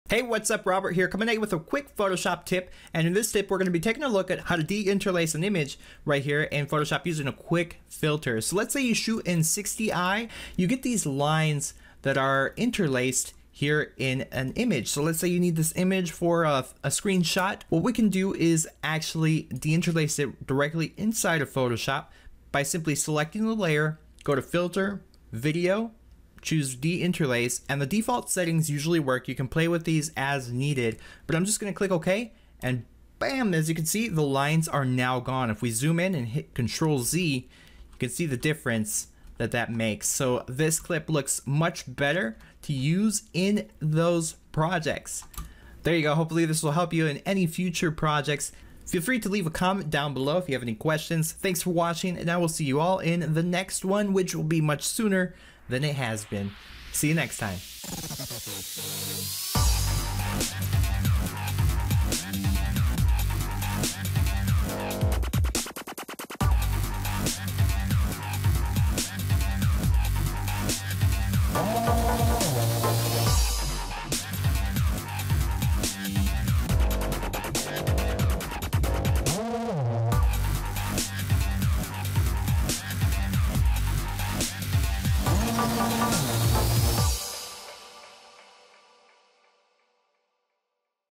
How to Deinterlace in Photoshop
Learn how to de-interlace images in this quick Photoshop tip!
Interlaced and Progressive Frame Rates Explained!

Useful for camcorder snapshots or screenshots taken from interlaced video.